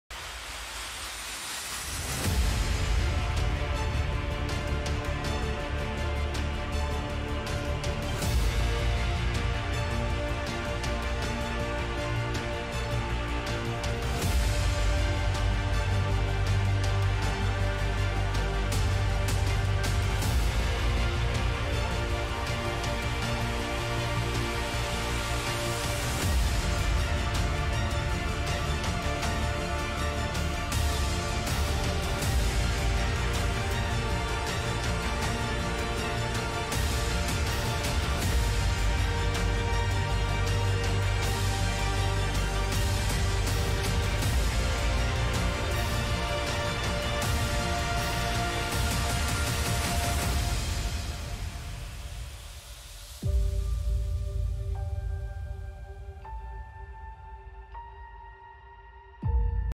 ESU comes from behind to down #22 Bloomsburg, 3-2.
ESU overcame a 2-0 halftime deficit to nationally-ranked Bloomsburg to down the Huskies, 3-2 on Sept. 22, 2021. ESU picked up its first win over a ranked foe in nearly 10 years.